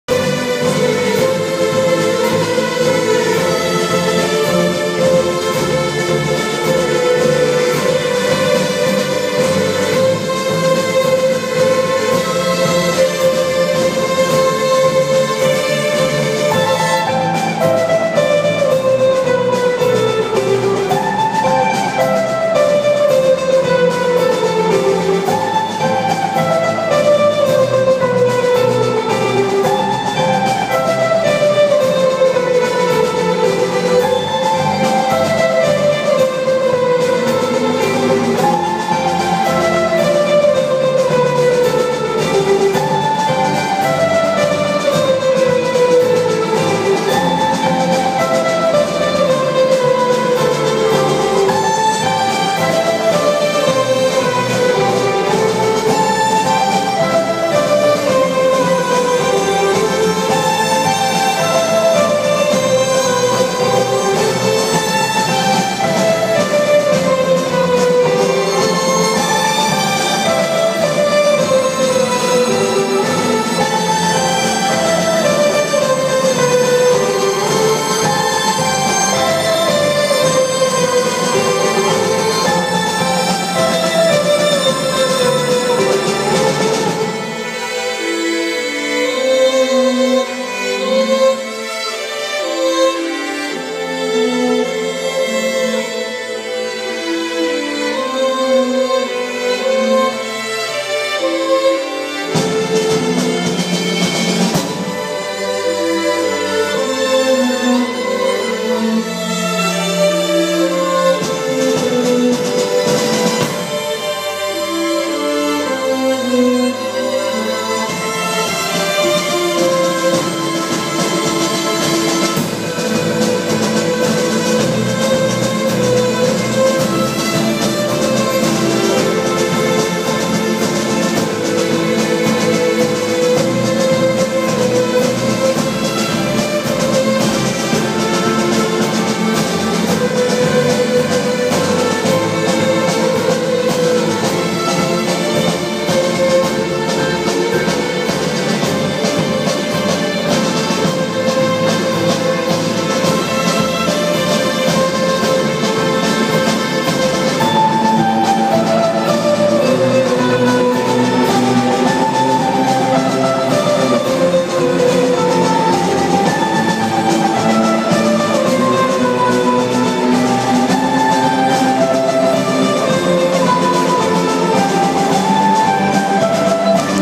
41 Strings at Sydney Opera House for Sydney Festival
for what's cool in Sydney and Global arts. 41 Strings featured as part of Sydney Festival 2012